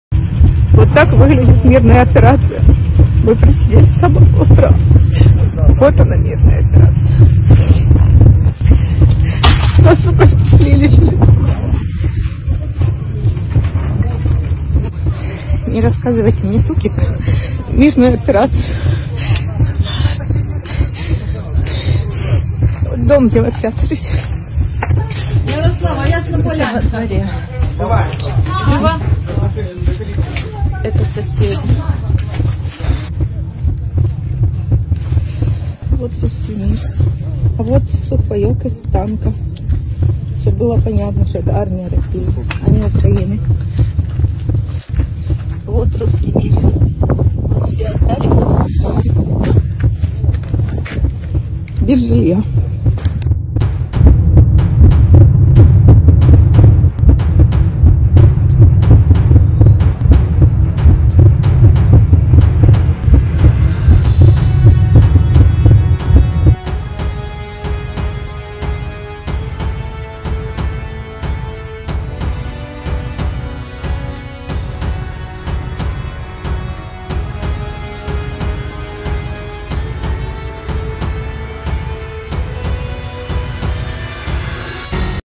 Украина сегодня.
Це не «операція визволення» - це війна! Це знищення людей!
Росіяни, не мовчіть!
Нам дуже треба ваша підтримка зараз !!!
Это не «операция освобождения» – это война! Это уничтожение людей!
Русские, не молчите!
Нам очень нужна ваша поддержка сейчас!!!